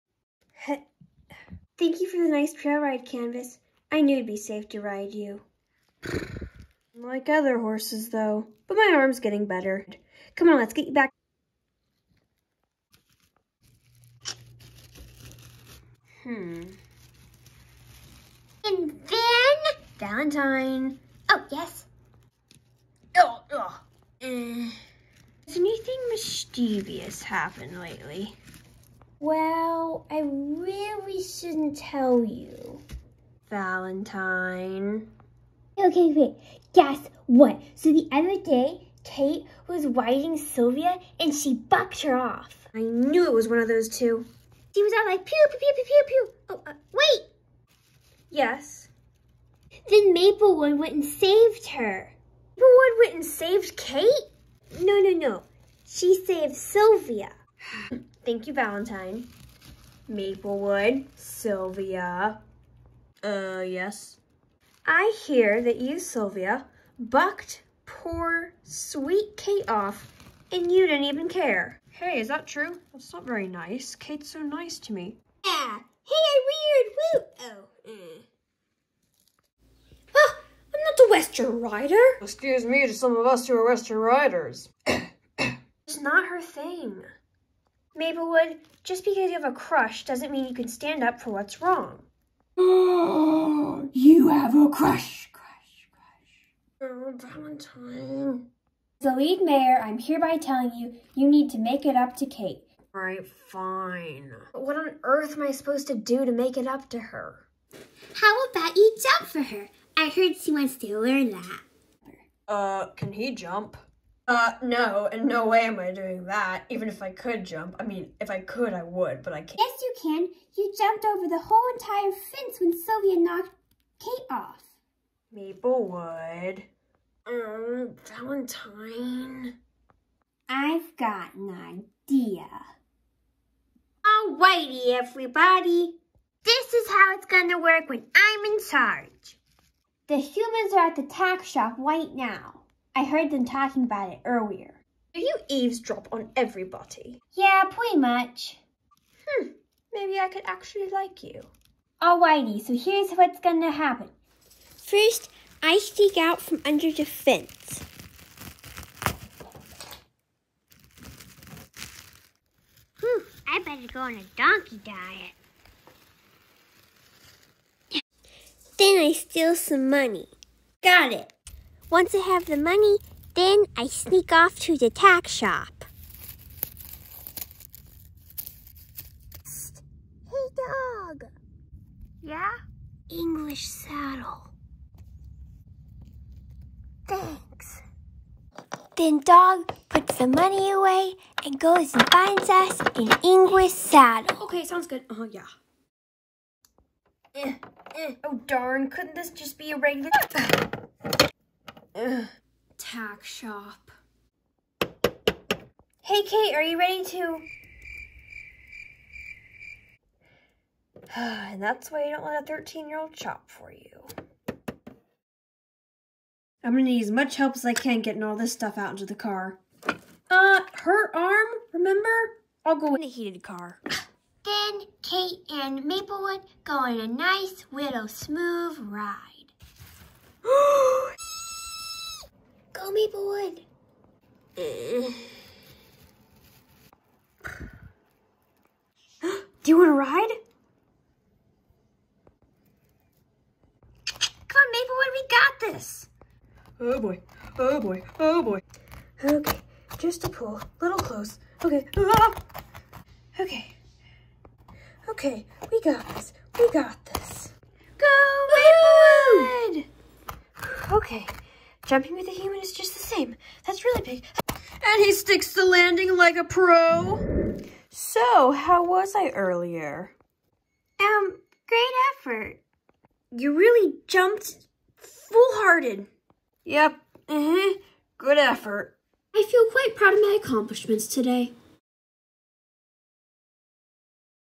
Schleich Misty Mountain Meadows S:1 E:3
Kate tries jumping, Valentine has an adventure, and Maplewood makes up for what he did!